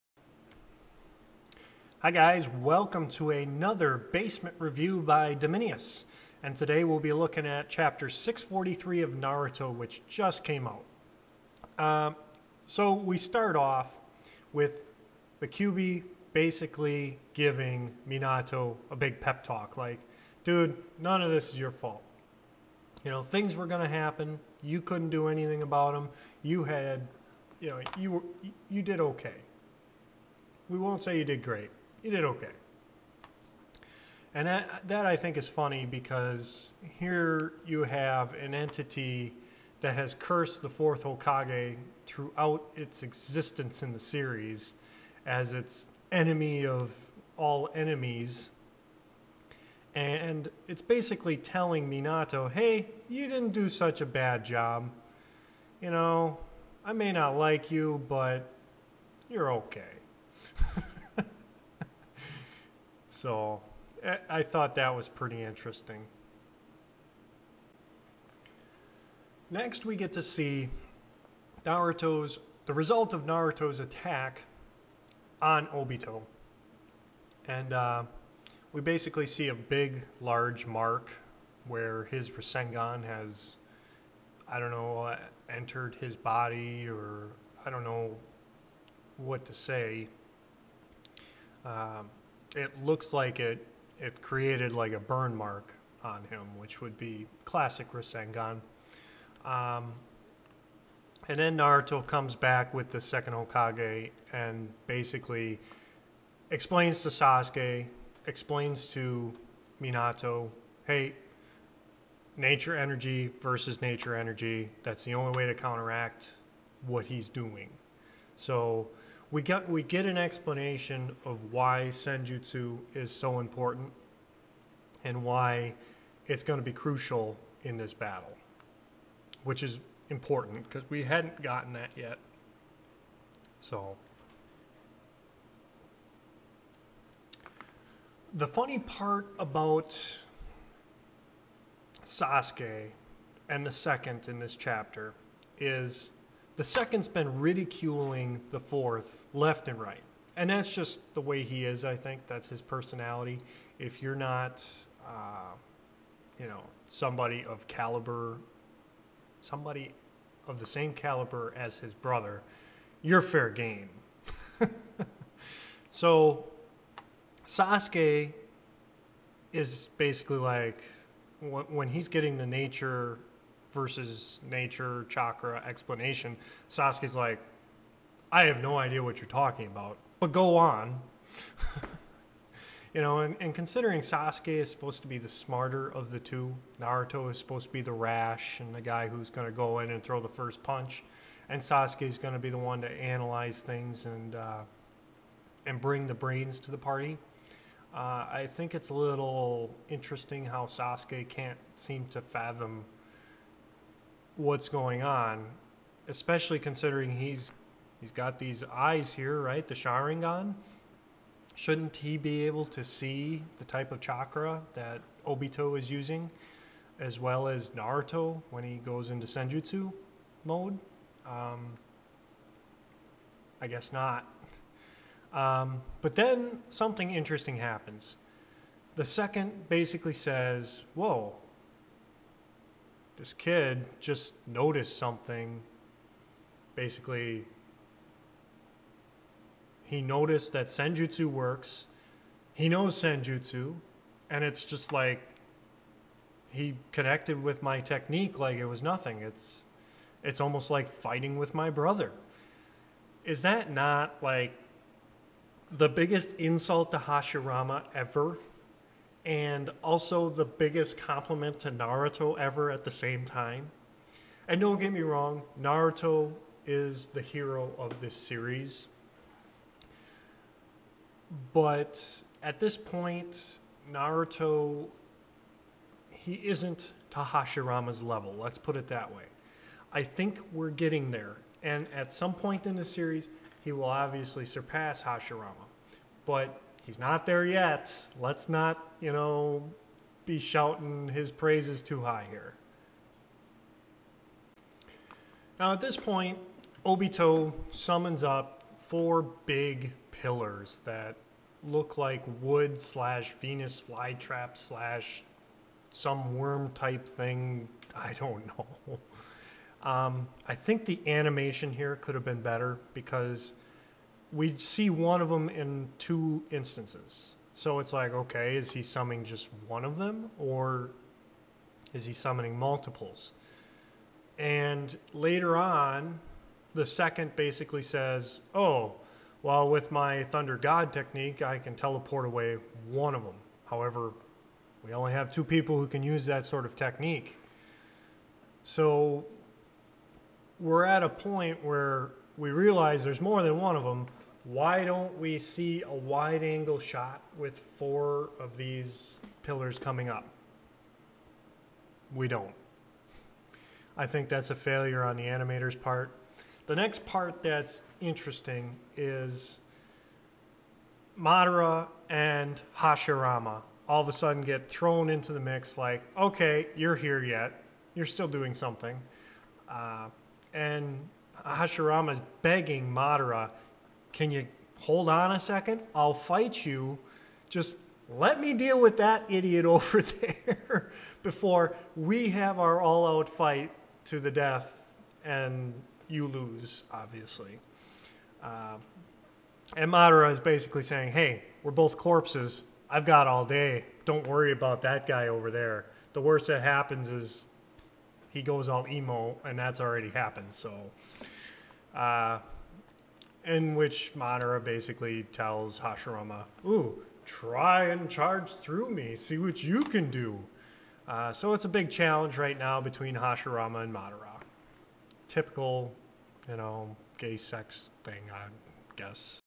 Basement Review: Naruto 643
Dim takes a look at Naruto Chapter 643, and give his first reactions to it.